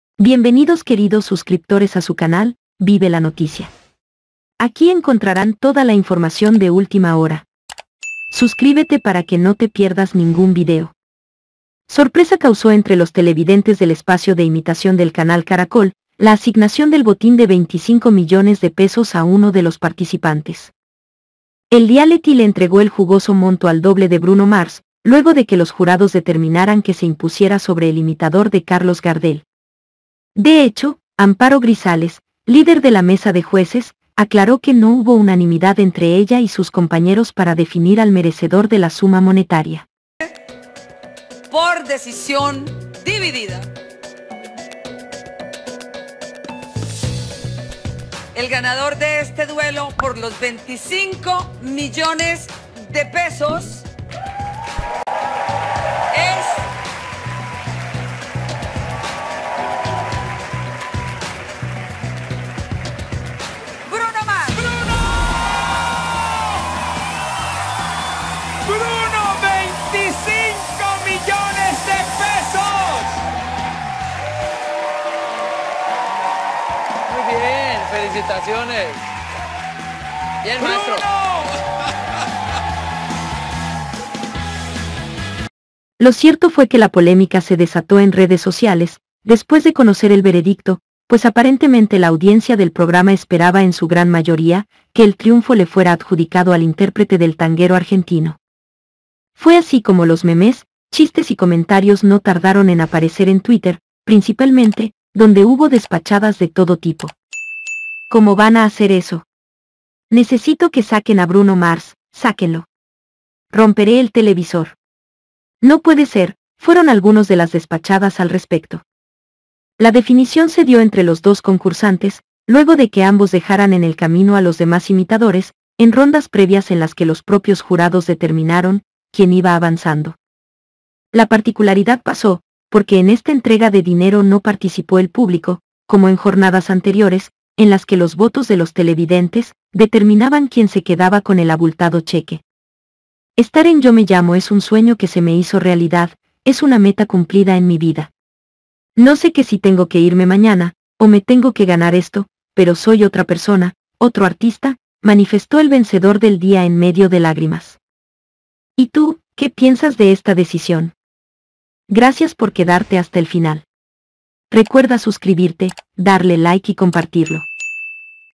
PASÓ HOY! JURADO DE "Yo me llamo"! RECIBE FUERTES CRÍTICAS POR SU DECISIÓN !HAY MUCHA DECEPCIÓN!
Sorpresa y decepción causó entre los televidentes del espacio de imitación del Canal Caracol, la asignación del botín de 25 millones de pesos a uno de los participantes.
Entérate aquí...!